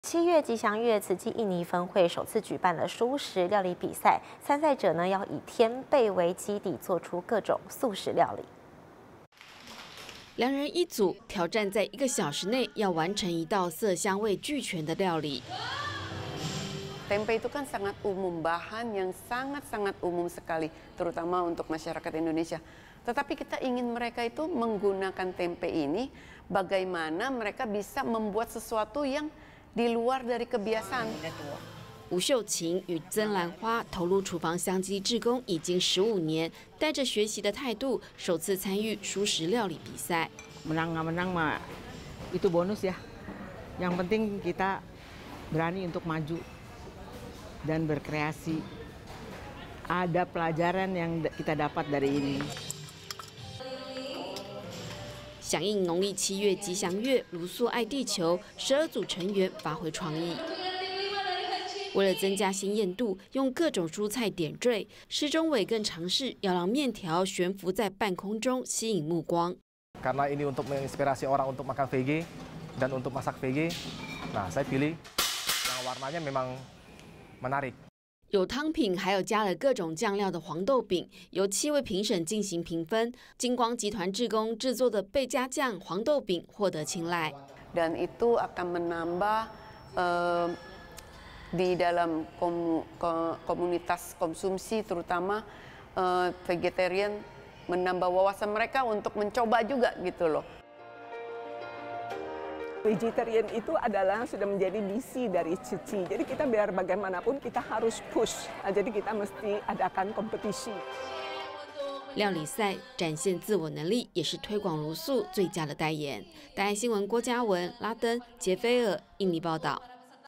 天貝料理神廚競賽 平凡食材推素很簡單｜大愛新聞
七月吉祥月，慈濟印尼分會，首次舉辦蔬食料理比賽，參賽者必須以天貝為基底，做出各種素食料理。